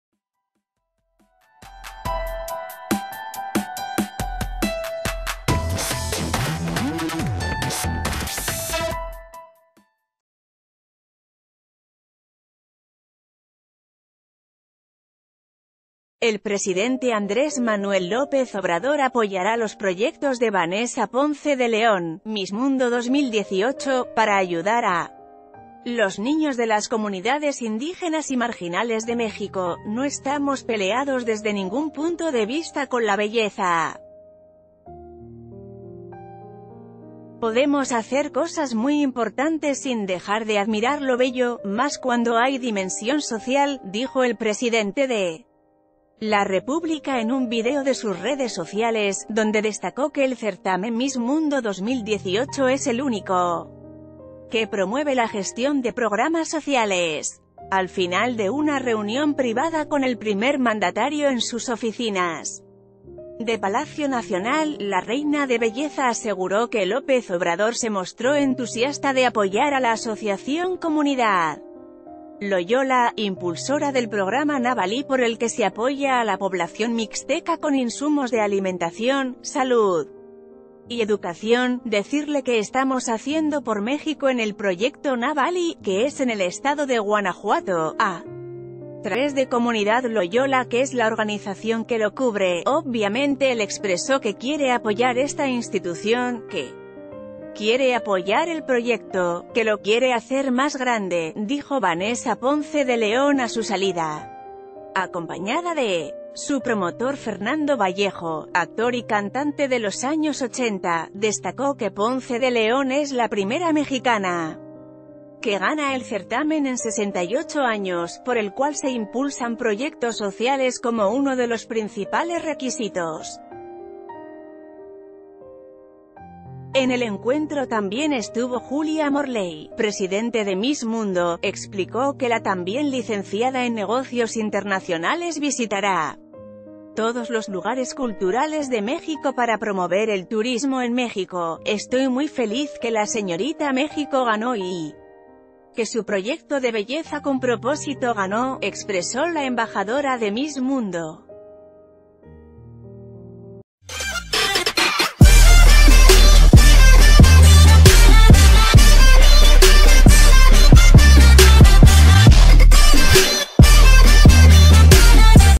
AMLO apoyará proyectos de Vanessa Ponce de León, Miss Mundo 2018
El presidente Andrés Manuel López Obrador apoyará los proyectos de Vanessa Ponce de León, Miss Mundo 2018, para ayudar a los niños de las comunidades indígenas y marginales de México.“No estamos peleados desde ningún punto de vista con la belleza, podemos hacer cosas muy importantes sin dejar de adm...